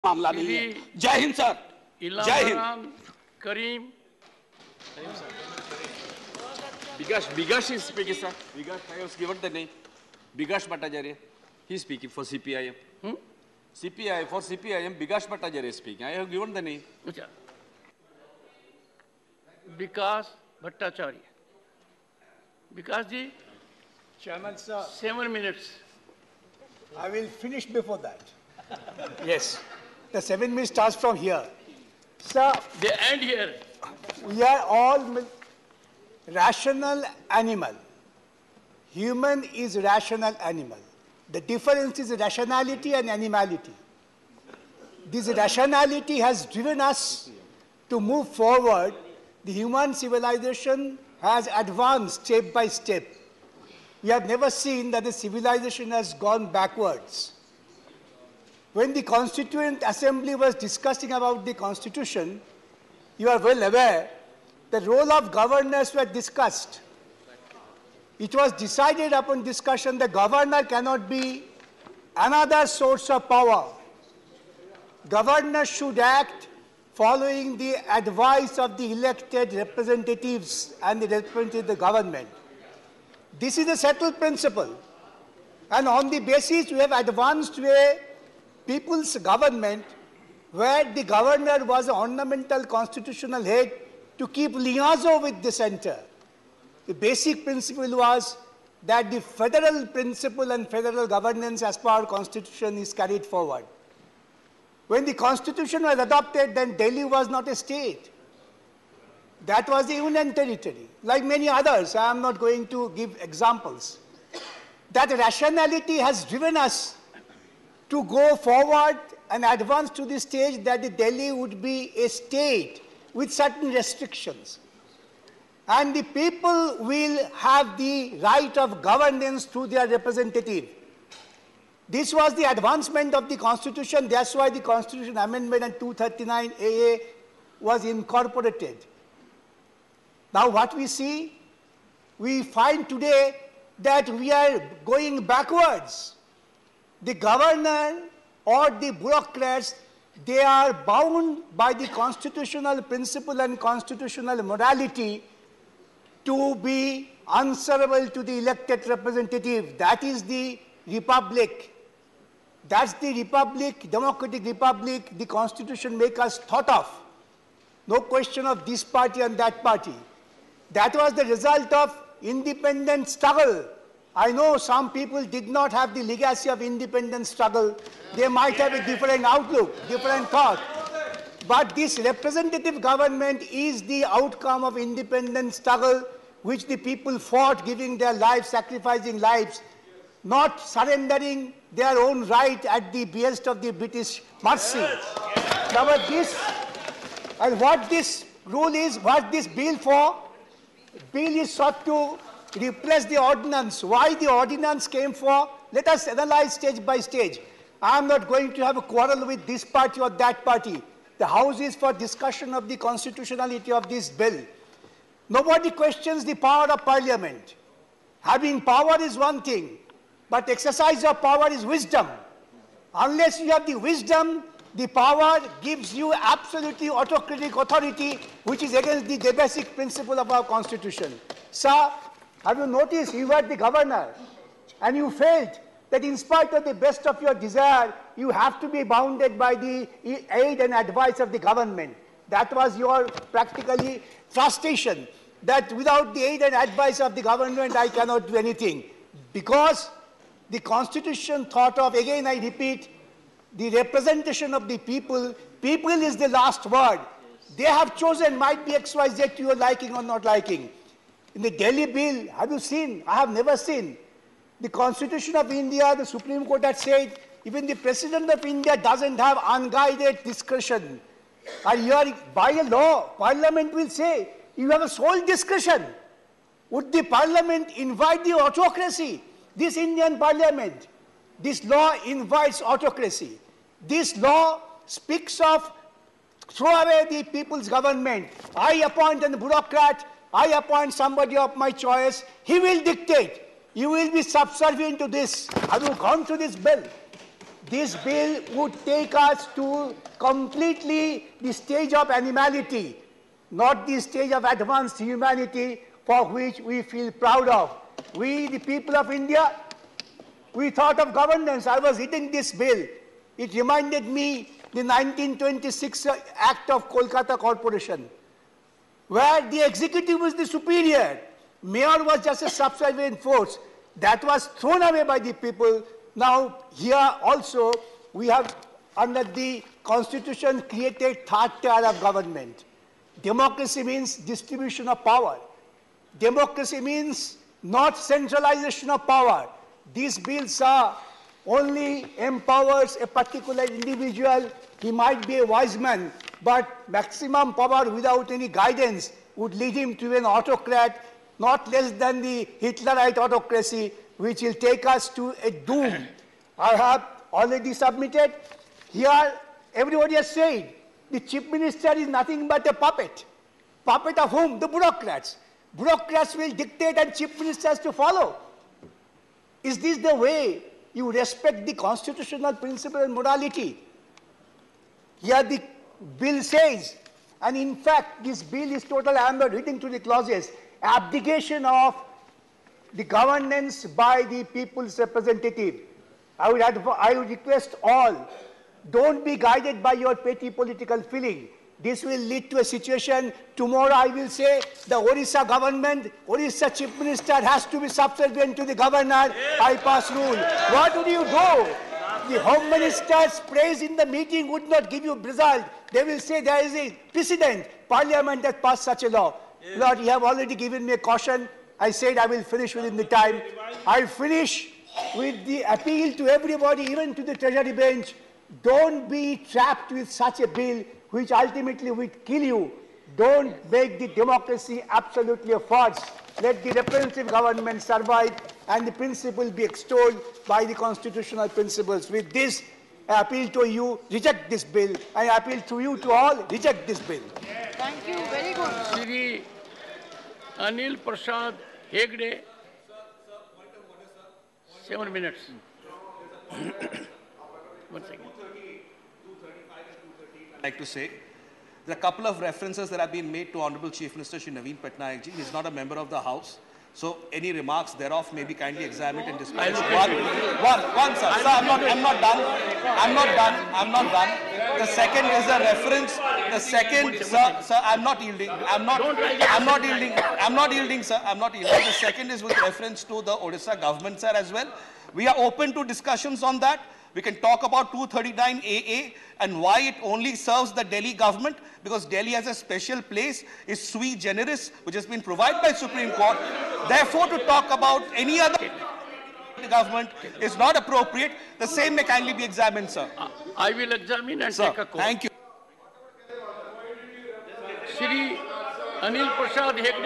Bikash Ranjan Bhattacharyya's Remarks | The Government of NCT of Delhi (Amend) Bill, 2023
Discussion on The Government of National Capital Territory of Delhi (Amendment) Bill, 2023 . The Bill further to amend the Government of National Capital Territory of Delhi Act, 1991